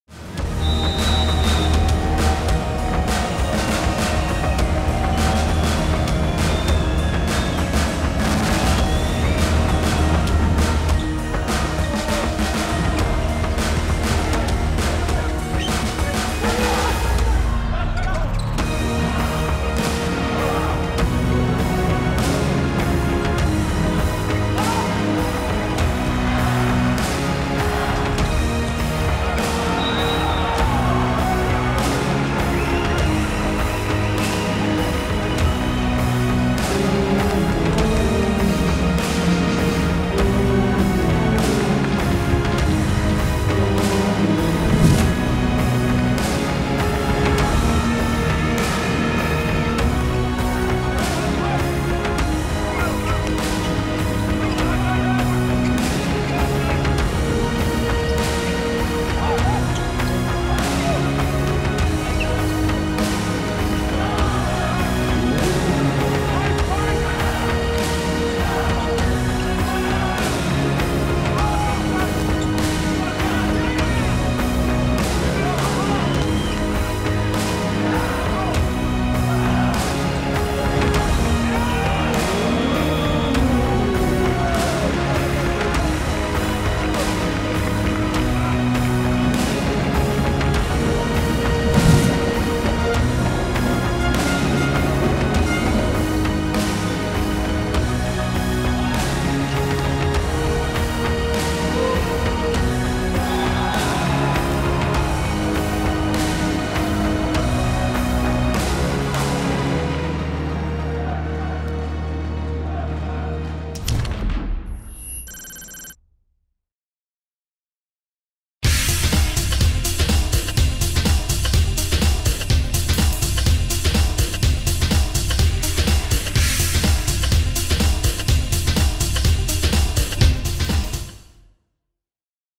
CLVolleyM | Top 3 Defences - 4th Round, Leg 1
Watch the selection of the best defences from the CEV Champions League Volley 2020 Men last leg!